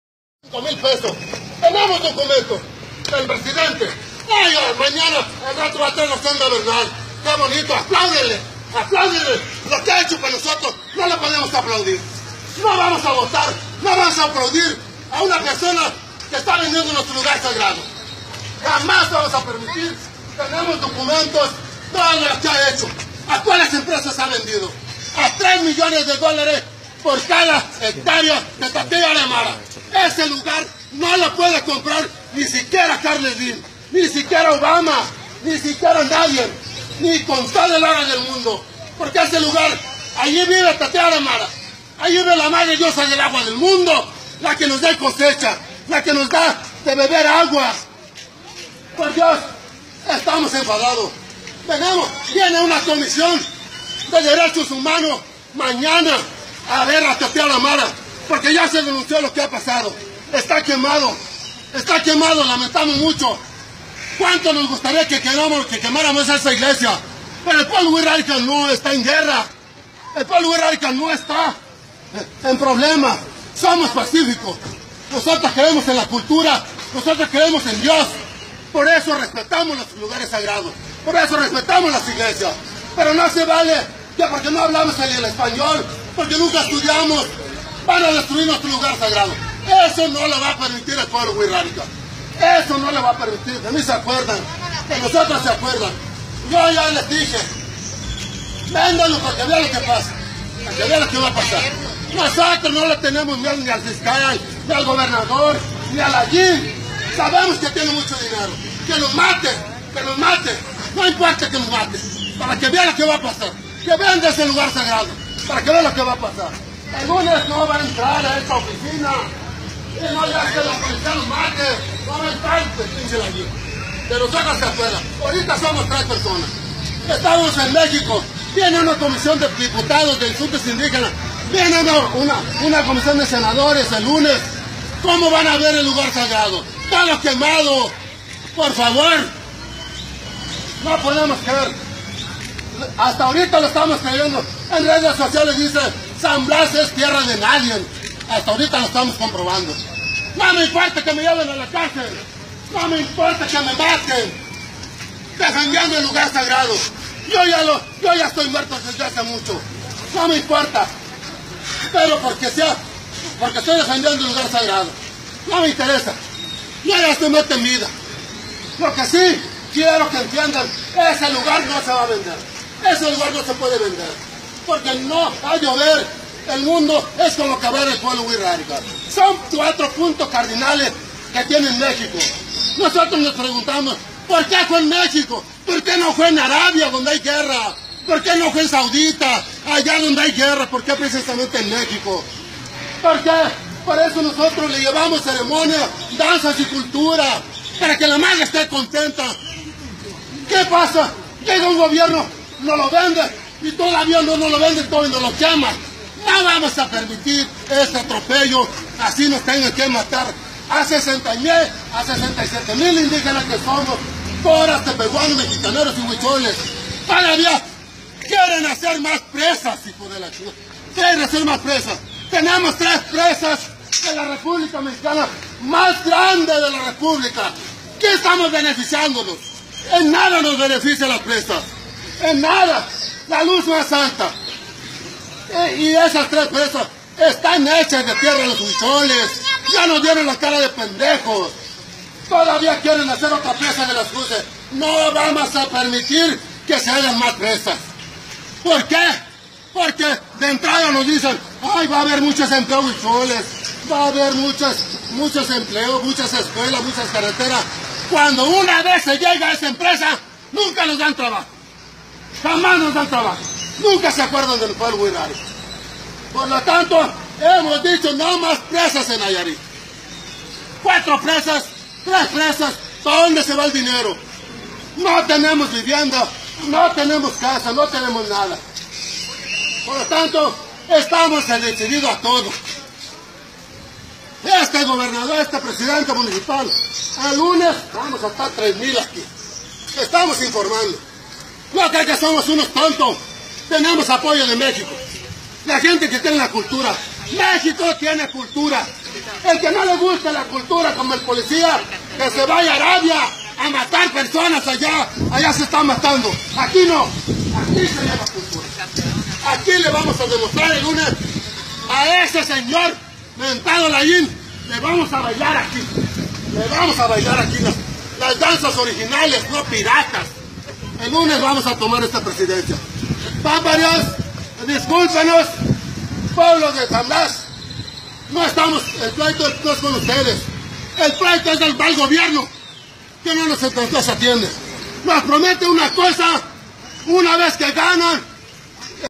Layín venderá tierra sagrada de indígenas el político "independiente" apoyado por el PRI.
Saca a relucir sus intenciones de vender a extranjeros los lugares que por años so. Hogares de los hermanos indígenas!
Comparte y ayuda!